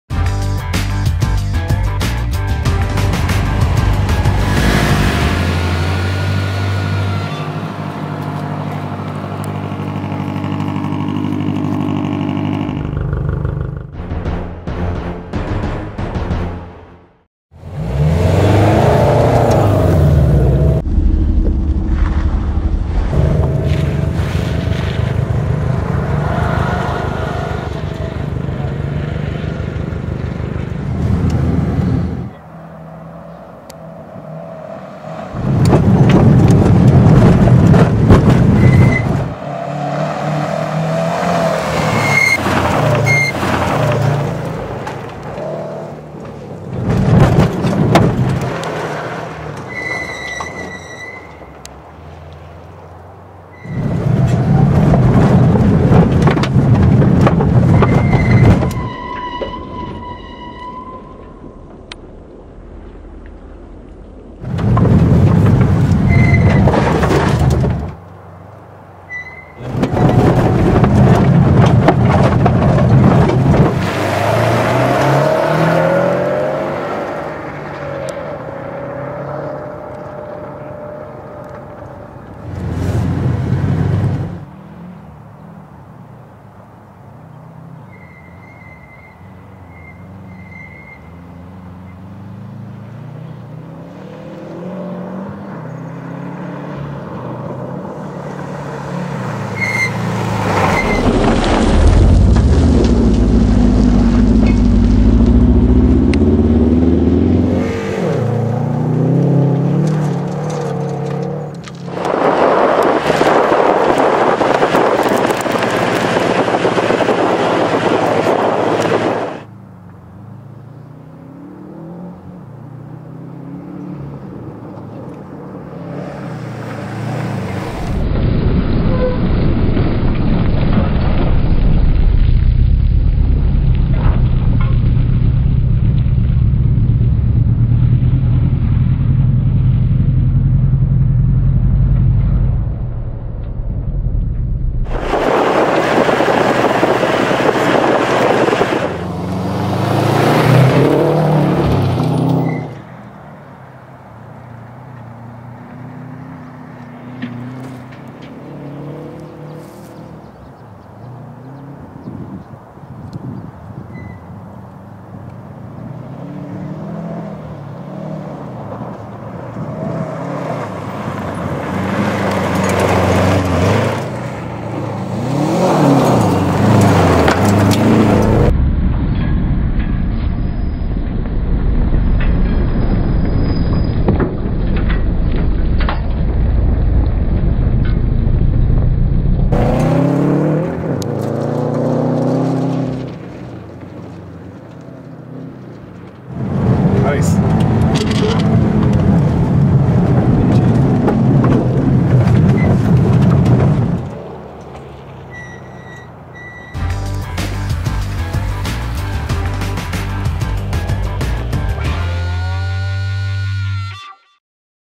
2011 Subaru WRX STI raw and unleashed
) Raw and unleashed : the new 2011 wide-body Impreza Subaru WRX and STI in Aspen, Colorado.